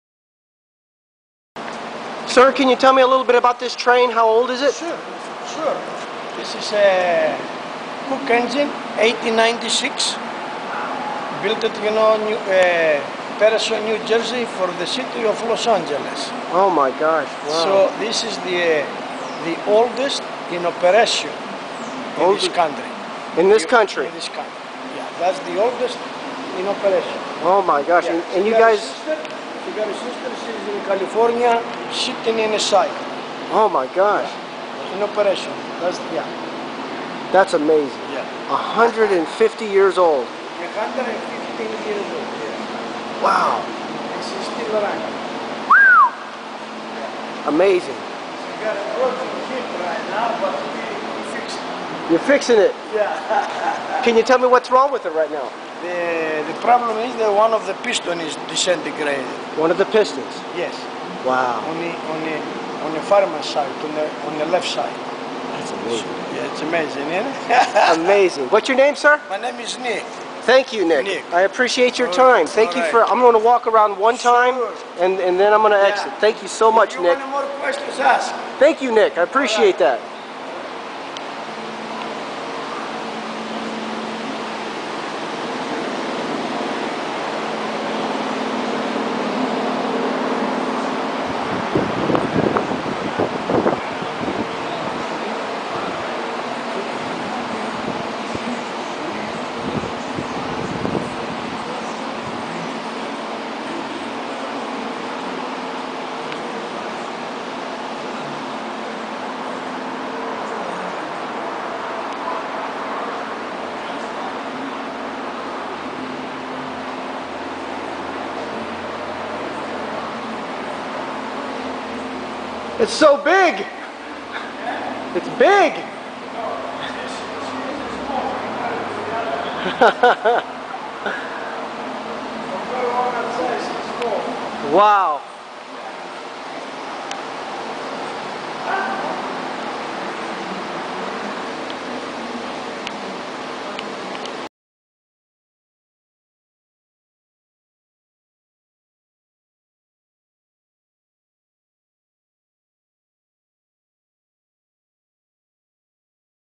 OLDEST OPERATING TRAIN IN USA / STEAM LOCOMOTIVE PUFFY # 2248 IN REPAIR SHOP
During my lunchbreak , I headed over to the Grapevine Train Depot , where I met Nick .. He works for the city of Grapevine , and was kind enough to let me in the train garage for a look . He says this is the oldest train in the USA that is still in operation ..115 Years Old ( in the video I misunderstood him , and say 150 ) oops ... Wow , this was a great day.. Thank You Nick !!